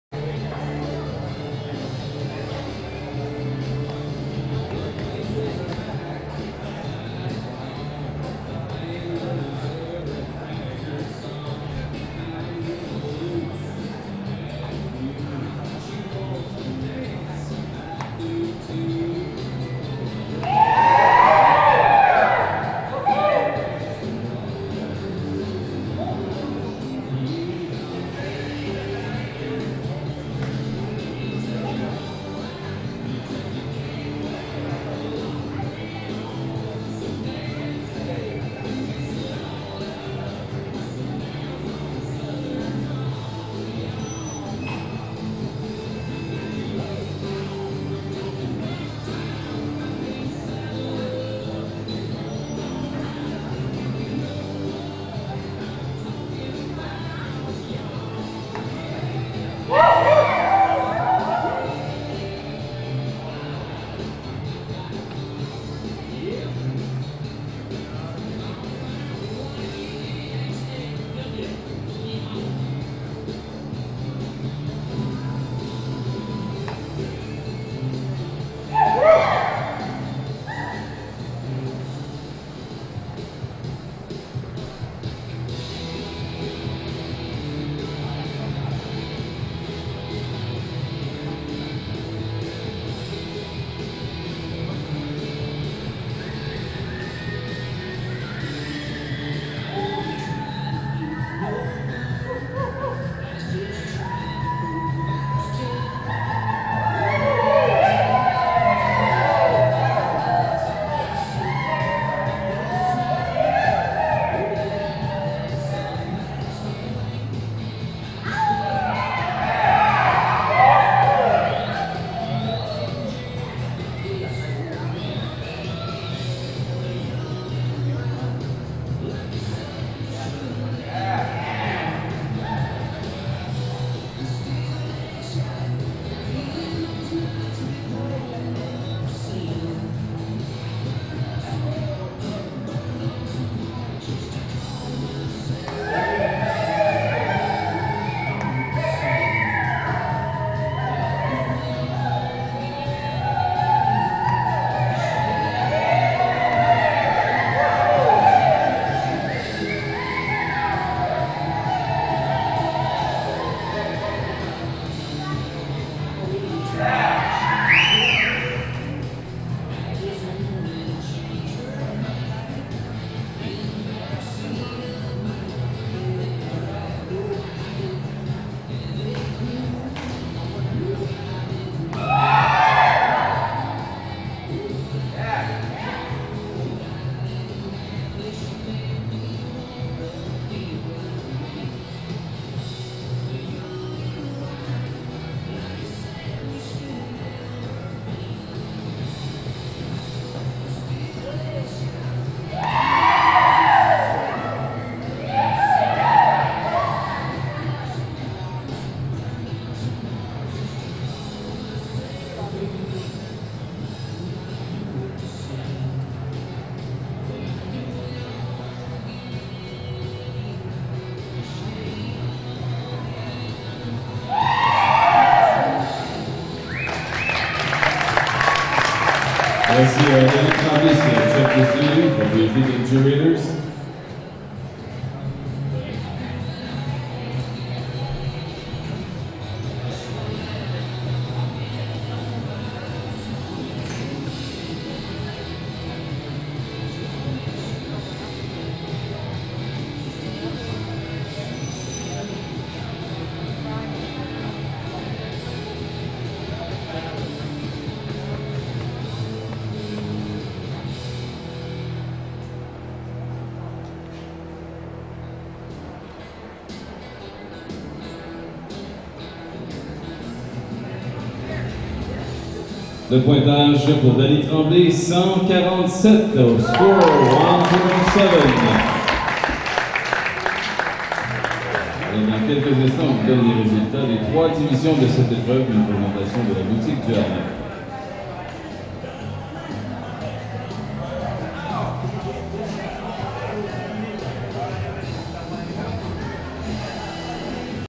Check This Dually
Dany Tremblay and Check This Dually riding the highest score of the AQR Futurity weekend